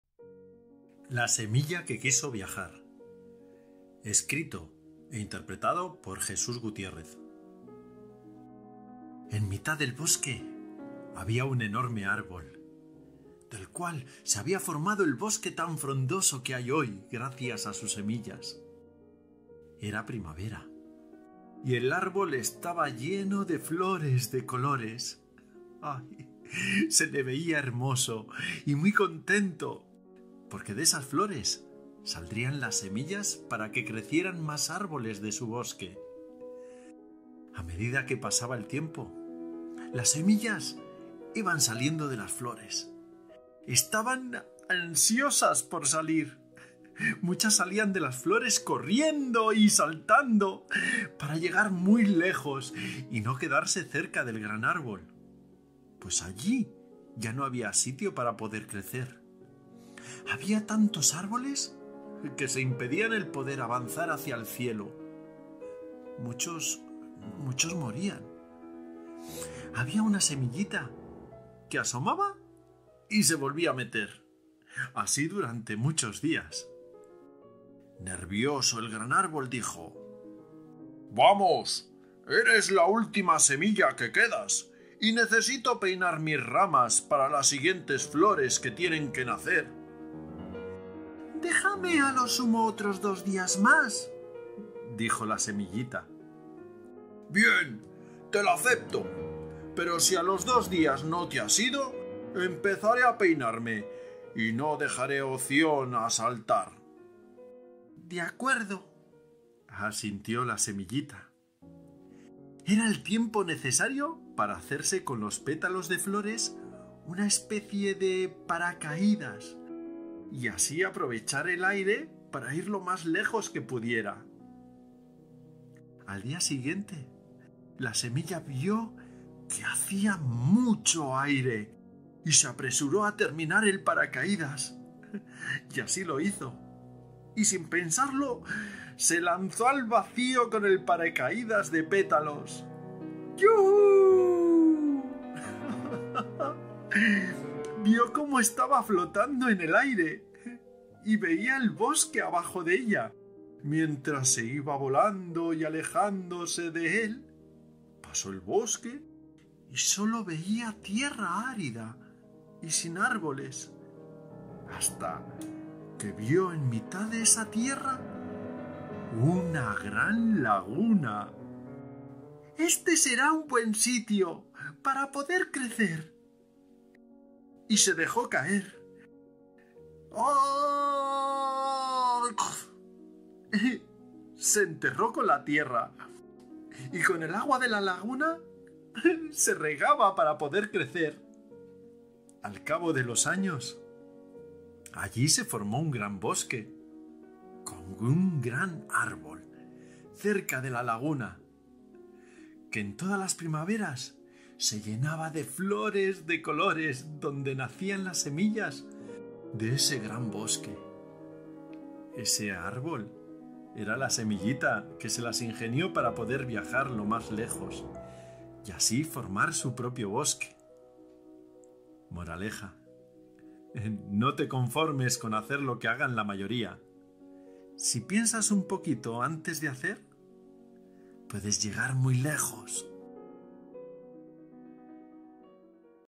CUENTOS - Jesús Gutiérrez | Taller Actor/actriz radiofónico U.P. de Palencia / Se Tú Creando
Trabajo personal, gracias a la formación radiofónica y teatral de la mano de Concha Santiago.